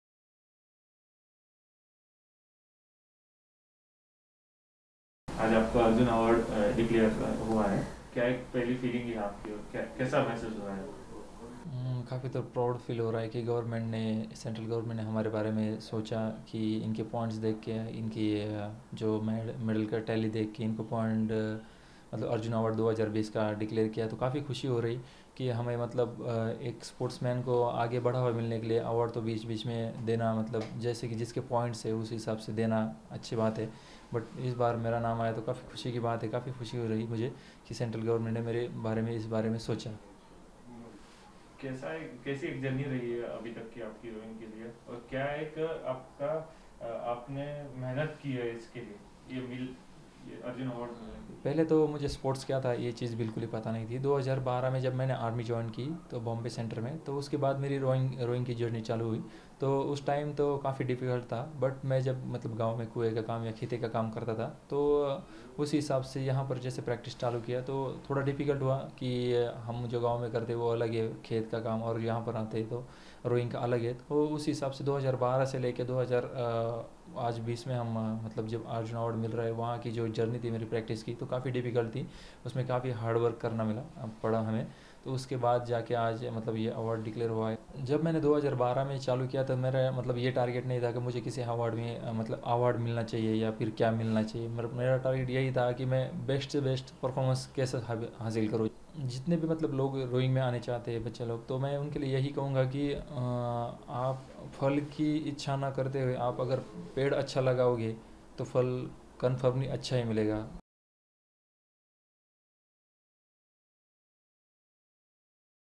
Getting Arjuna Award is ‘proud moment for me’: Dattu Bhokanal
Pune (Maharashtra), Aug 29 (ANI): Indian rower Dattu Bhokanal who will receive prestigious Arjuna Award expressed his joy by saying that it is a proud moment for him and he is delighted. “I feel very proud. I am delighted. Giving an award to a sportsperson, to encourage them further, is good. When I started in 2012, it was never my target to win an award. All I aimed at, was to give my best performance.”